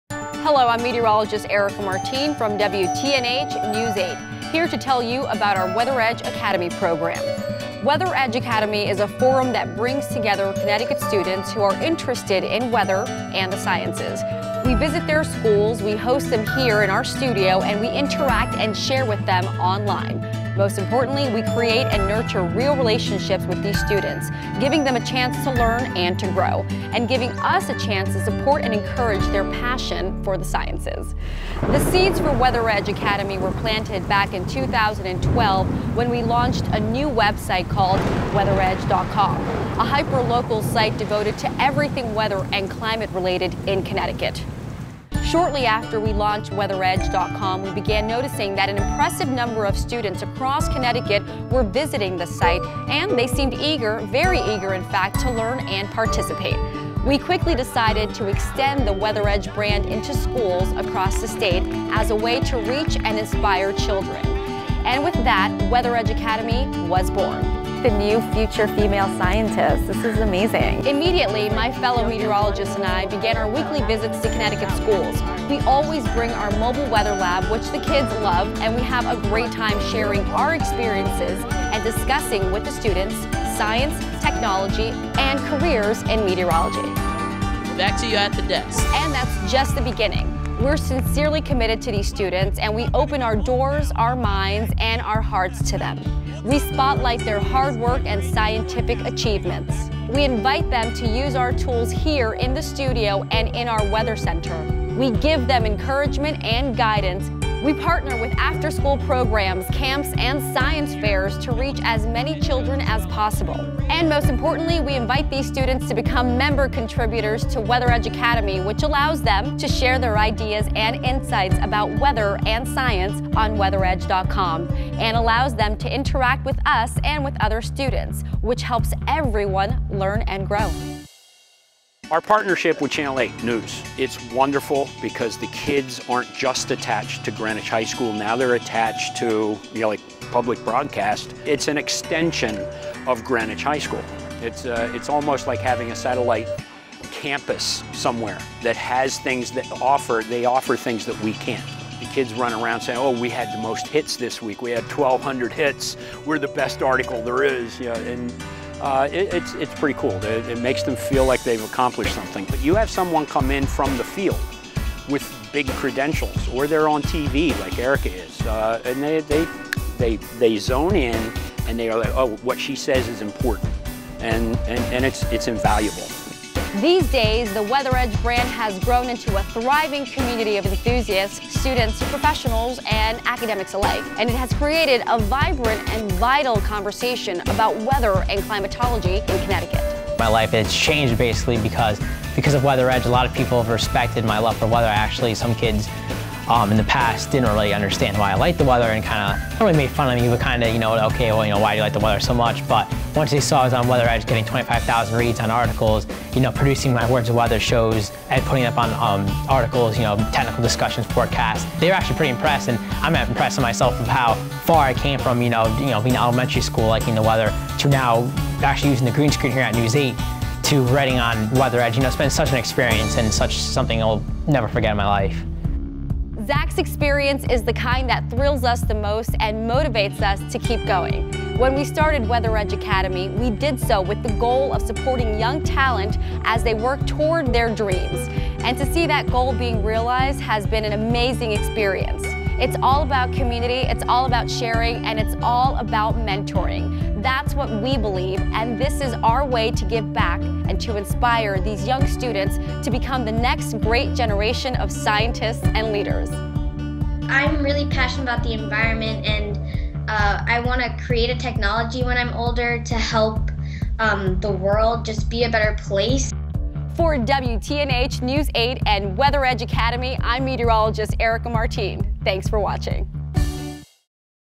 WXedge Academy
A presentation that describes WTNH's committment to the community and education through its weather microsite WXedge.com.